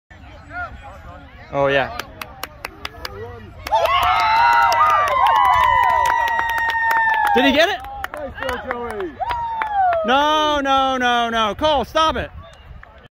Cole's first touchdown in flag football 7U league.
Cole scores his first touchdown in the Gateway flag football league.